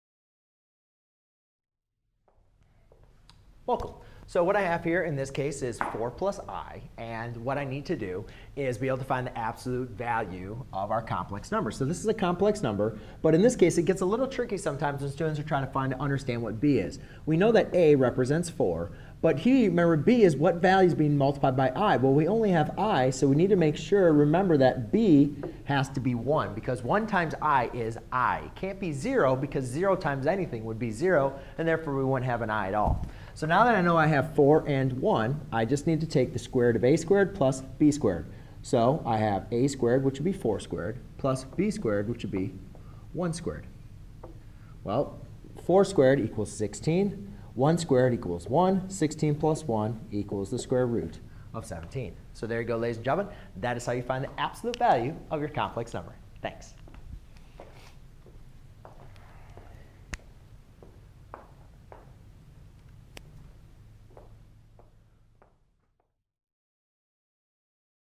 Tutorial - Finding the absolute value of a complex number ex 5, (4 + i)
In this video series I will show you how to find the absolute value of a complex number.  The absolute value of a complex number represents the distance from a complex number to the origin.  We will do this by taking the absolute value of the square of both of our real numbers from a complex number
(4 + i)